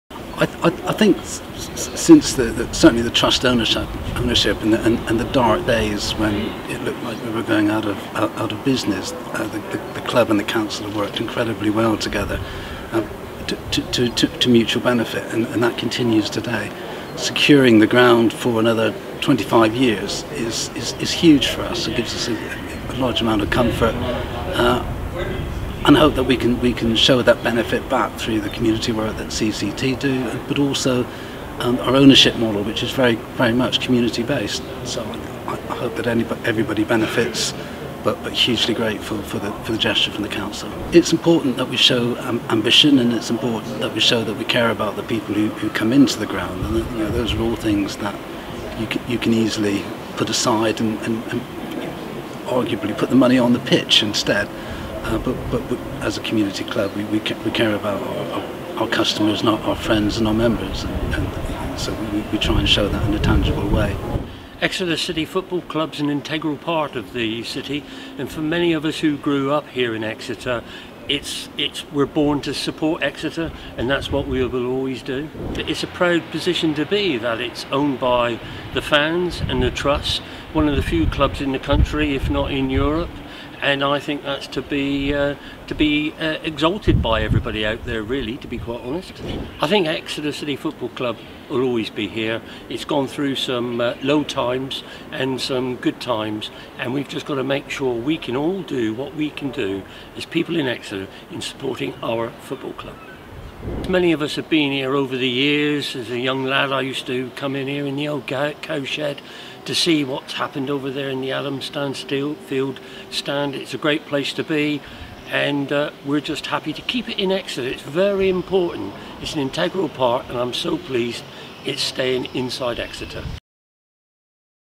Lease agreement secures Exeter City Football Club’s future for a further 25 years
Exeter City has signed a new lease securing its future at St James Park for a further 25 years.